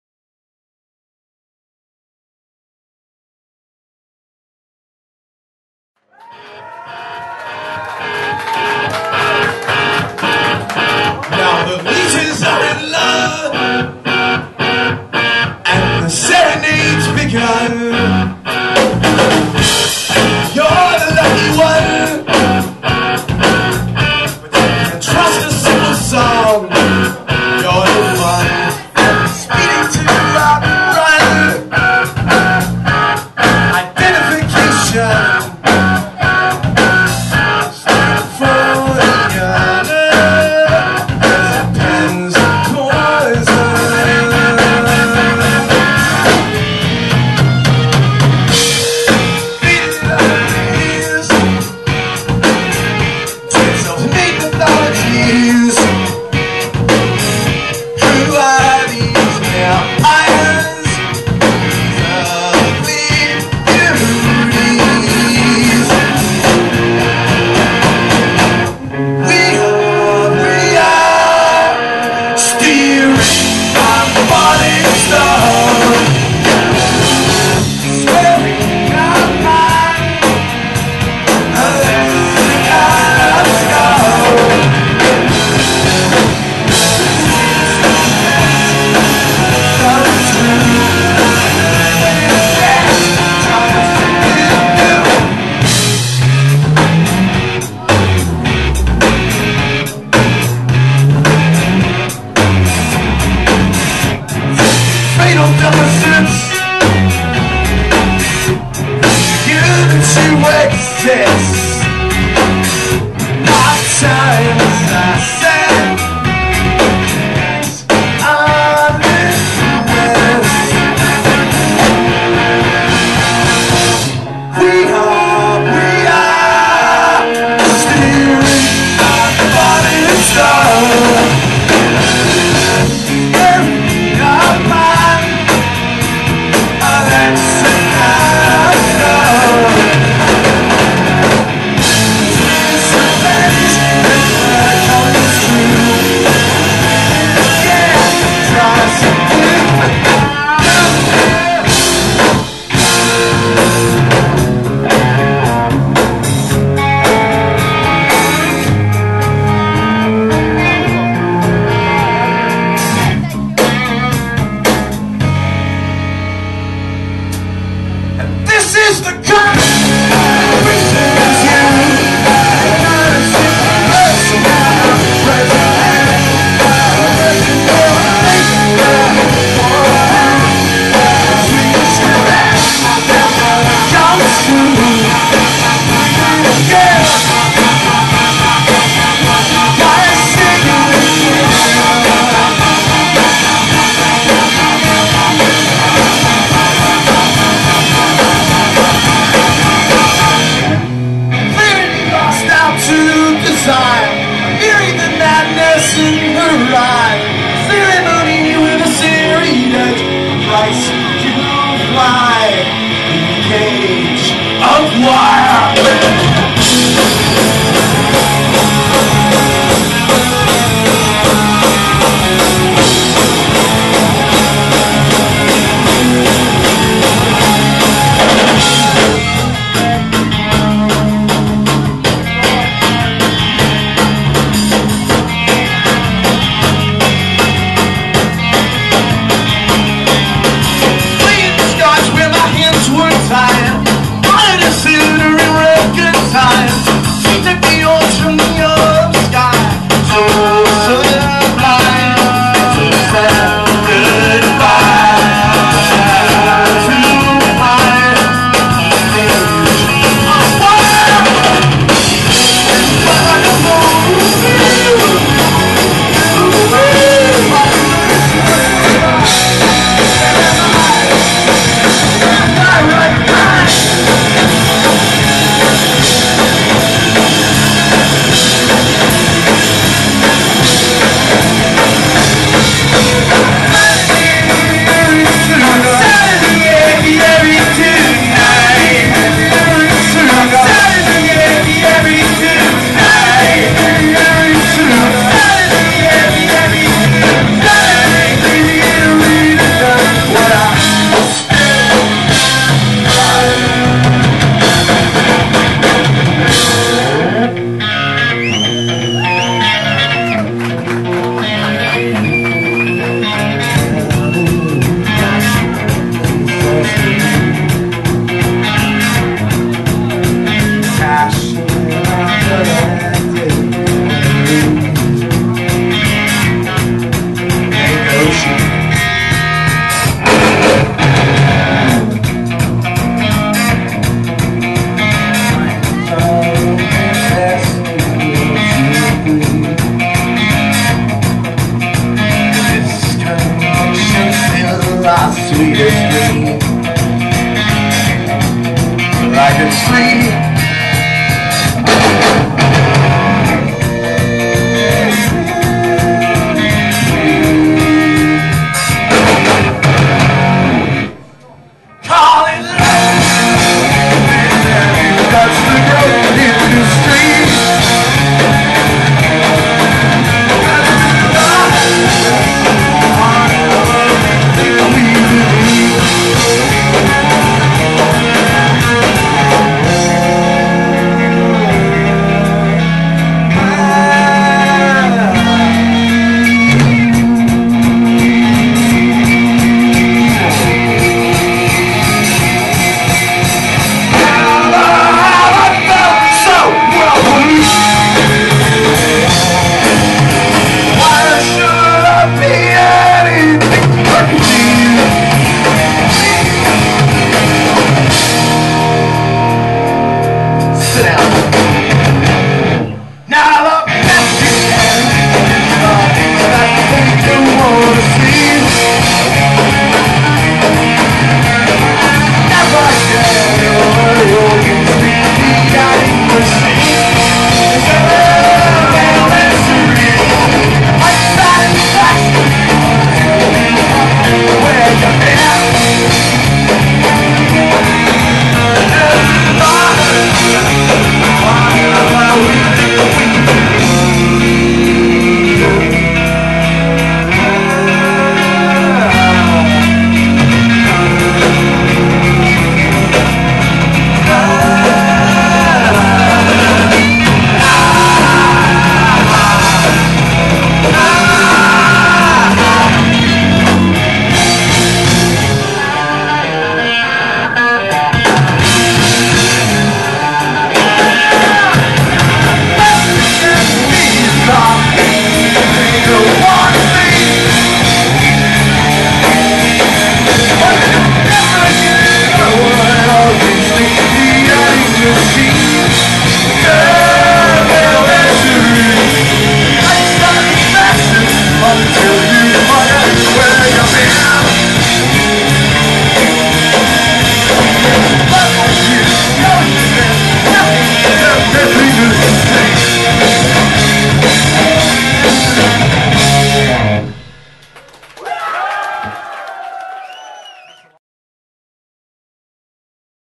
BURNING AIRLINES
Burning Airlines play Exeter Cavern around 2000/2001